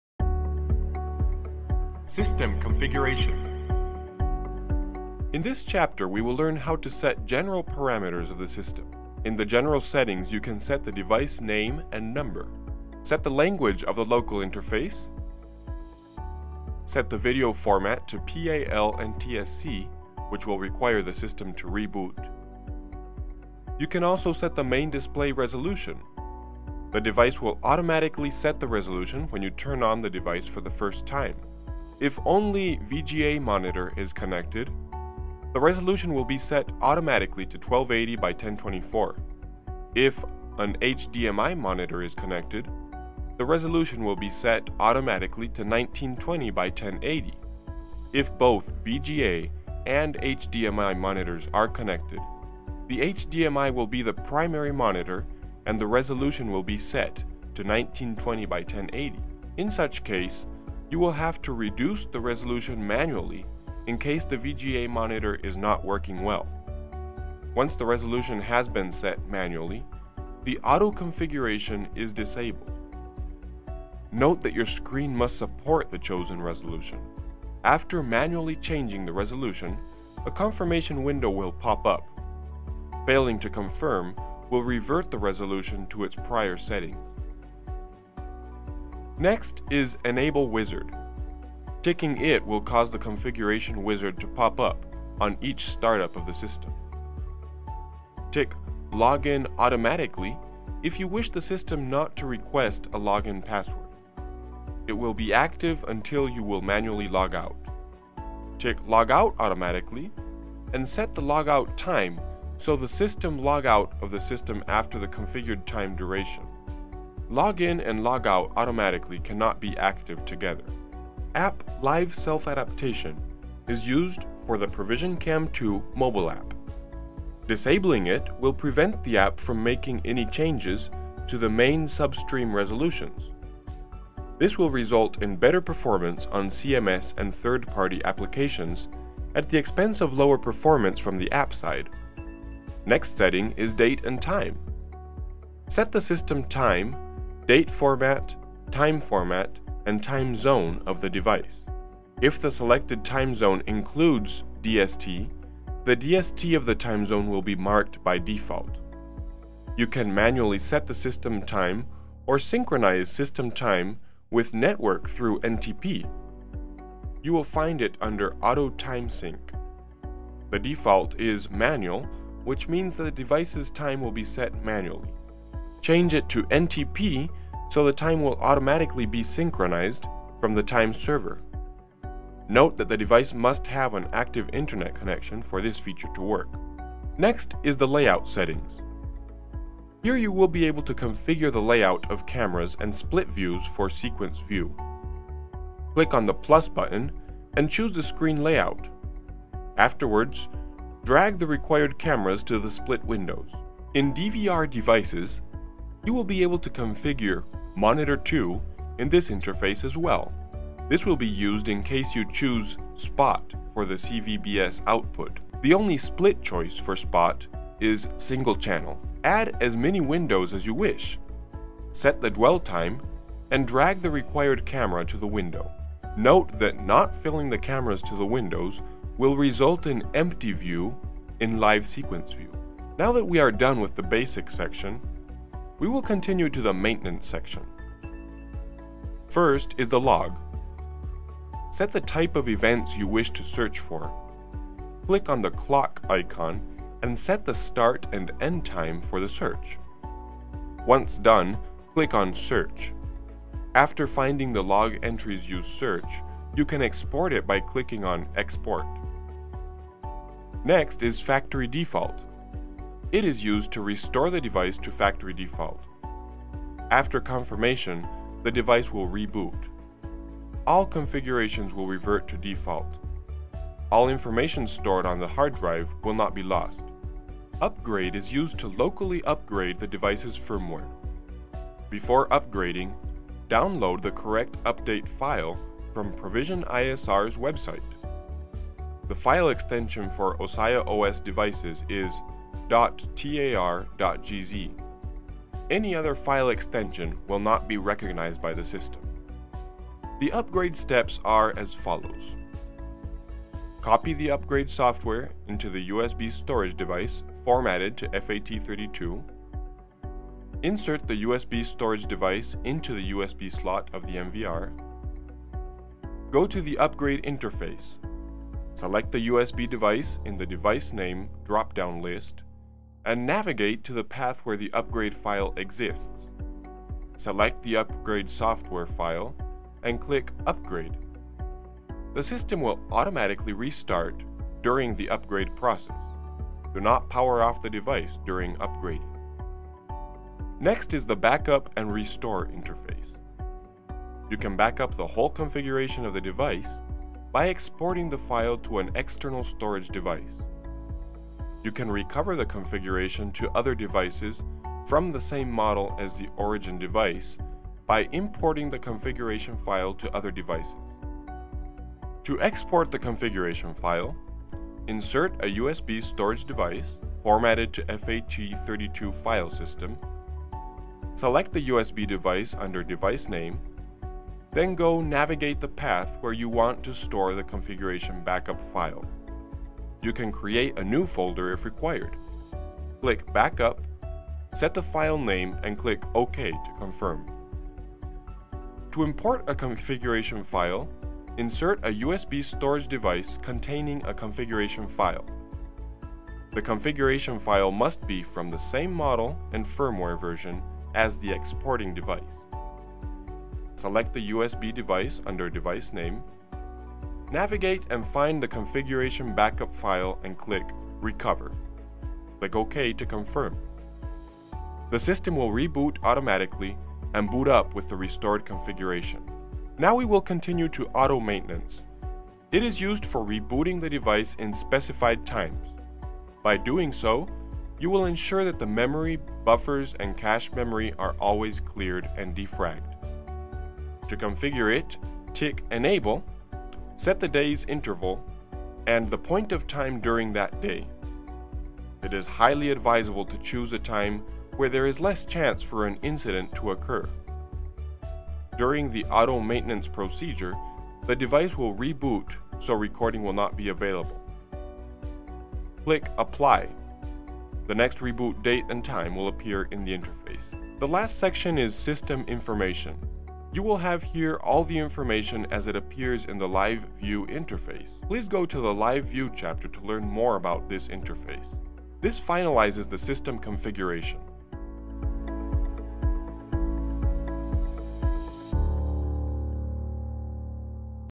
Ossia Chapter 11- System Configuration
DVR/NVR Ossia O.S interactive manual,
Chapter 11-  System Configuration